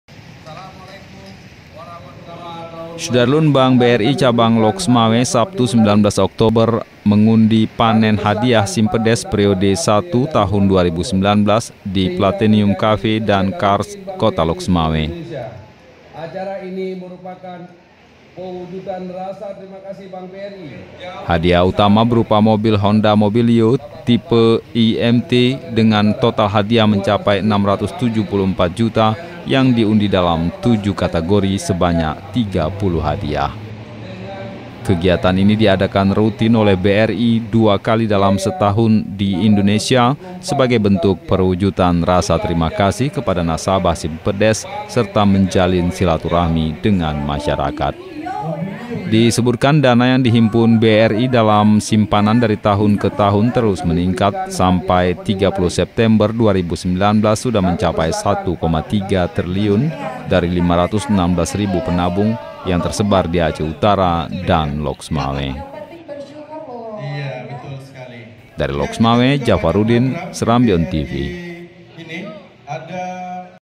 Undian Panen Simpedes BRI Lhokseumawe dengan Total Hadiah 674 Juta
SERAMBITV.COM, LHOKSEUMAWE – Bank BRI Cabang Lhokseumawe Sabtu (19/10), mengundi panen hadiah simpedes periode I Tahun 2019 di Platinum Café dan Cars Kota Lhokseumawe.

Hadiah utama berupa Mobil Honda Mobilio Tipe E MT, dengan total hadiah  mencapai Rp 674 juta yang diundi dalam tujuh kategori sebanyak 30 hadiah.

Kegiatan ini diadakan rutin oleh BRI dua kali dalam setahun di Indonesia, sebagai bentuk perwujudan rasa terimakasih kepada nasabah simpedes, serta menjalin silaturahmi dengan Masyarakat.

Disebutkan, dana yang dihimpun BRI dalam bentuk simpanan dari tahun ke tahun terus meningkat, sampai 30 September 2019 sudah mencapai 1,3 triliun dari 516 ribu penabung, yang tersebar di Aceh Utara dan Lhokseumawe.